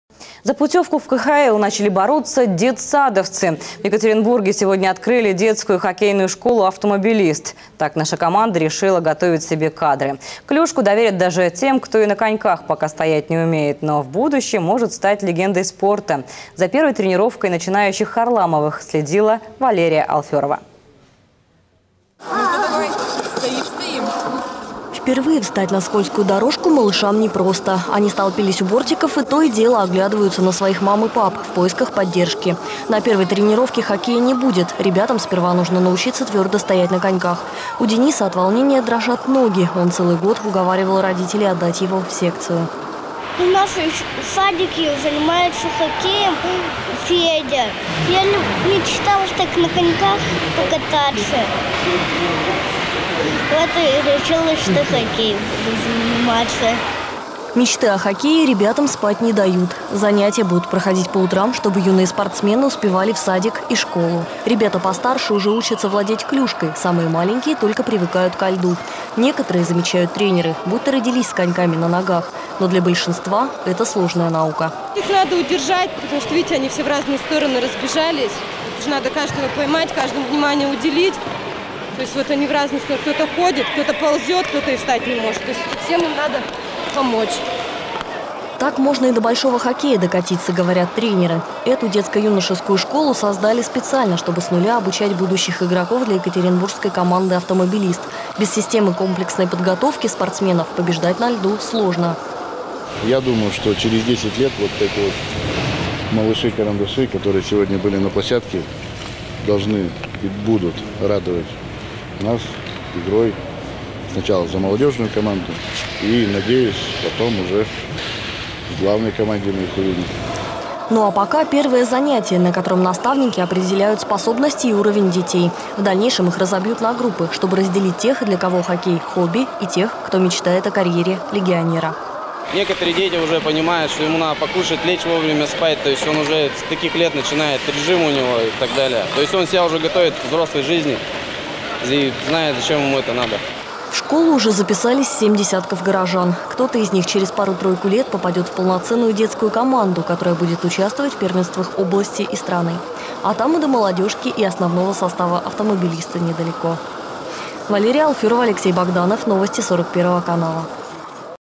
За путевку в КХЛ начали бороться … детсадовцы
В Екатеринбурге сегодня  открыли детскую хоккейную школу "Автомобилист"